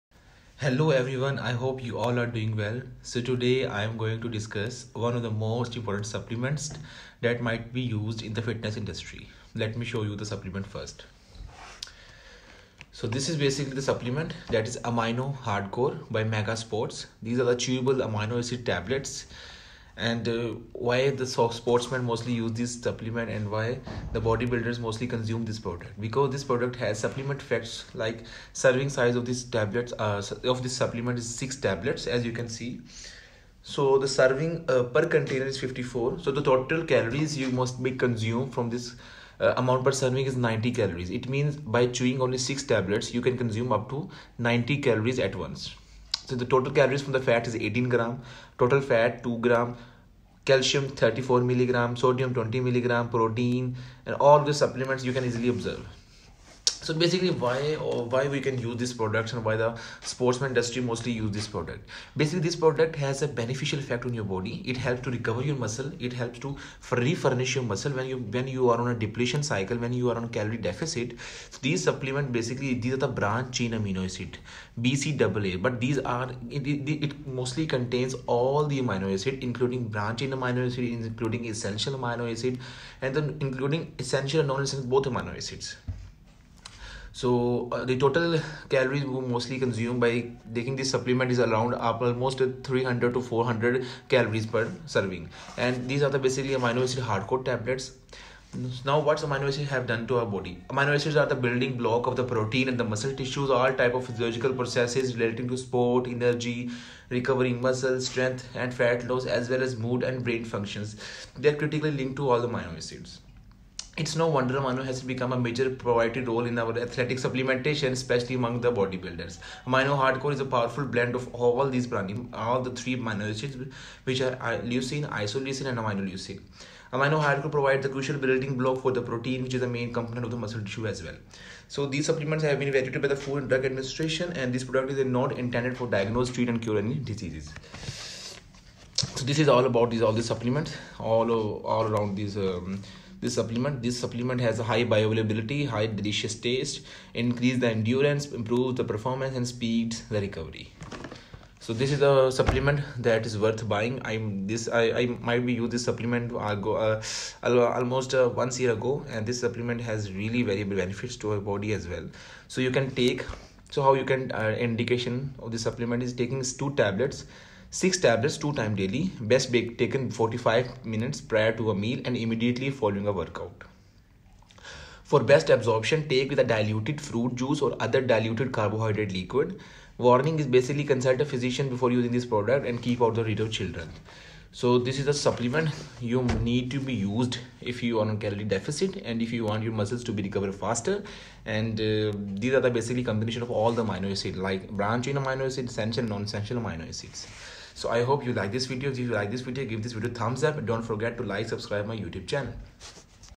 Amino Chewable tablet| Complete guide and review about Amino Hardcore Tablets.
Proper amino acid levels are important for muscle development and strength. They help control the balance between the atrophy and growth of human muscle. Supplementing your diet with essential amino acids may increase the supply of nitrogen to your body.